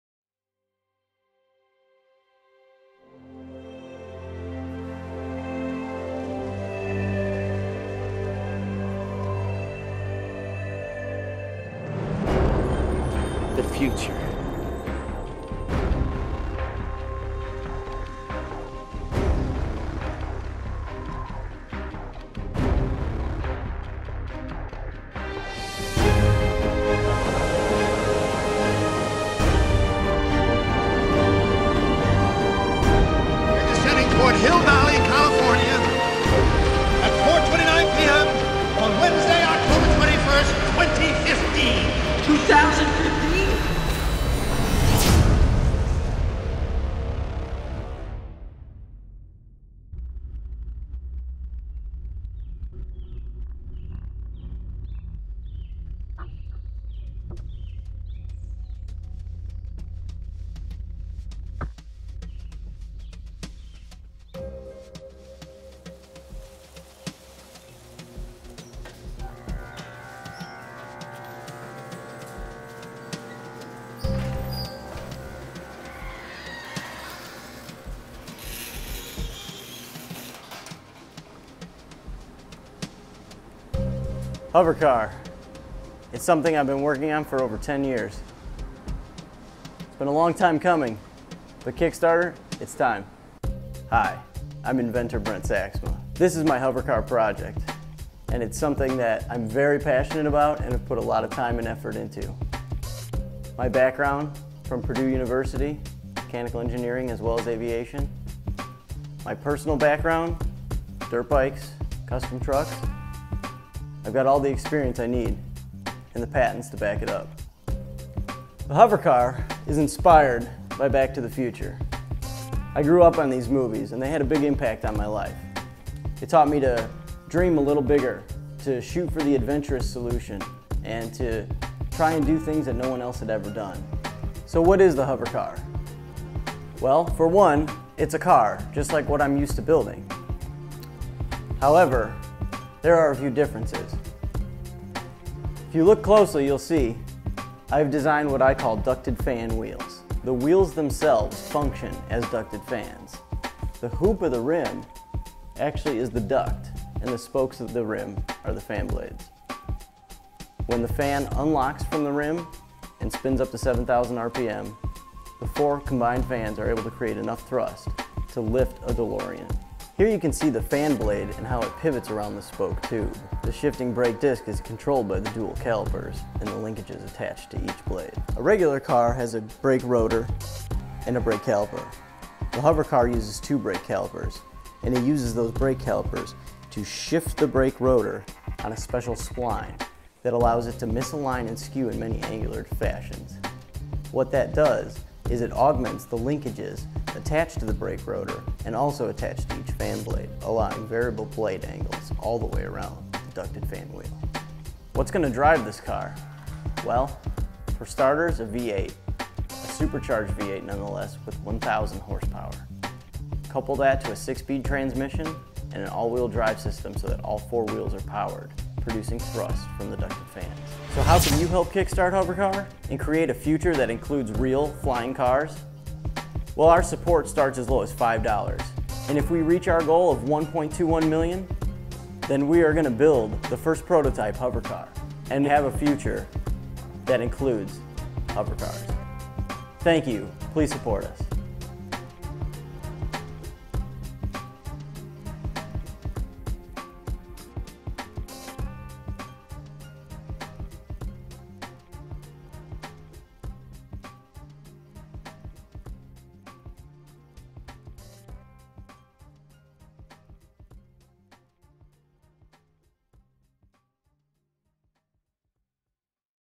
HoverCar™ - Kickstarter Campaign
HoverCar™ is the most daring attempt at a flying car that has ever been undertaken.  Inventor Brent J. Saxsma is taking to Kickstarter for the $1.21 million necessary to get this Supercar project off the ground - literally.  When successfully funded, construction will begin on the first prototype of this 1,000 horsepower Vertical Takeoff & Landing (VTOL) capable vehicle, utilizing Inventor Brent's multiple Patented technologies.  These technologies include the Ducted-Fan Wheels™ that utilize the Pin-Lock System™, the Variable-Blade-Angle-Brakes™ that ride on the Misalignment Spline™, and the Adaptive Control Arm™ Air Suspension.  Please help us Kickstart HoverCar™!